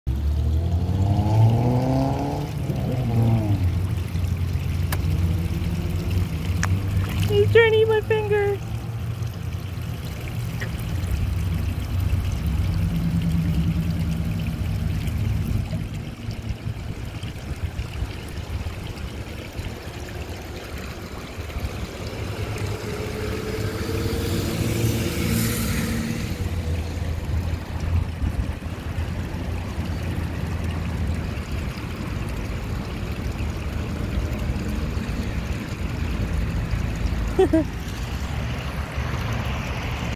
Munchy Koi
These big koi fish were nibbling on my fingers. Kinda felt like a weak chip-clip. They made suction noises. I wanted to buy them or at least feed them but the owner said no...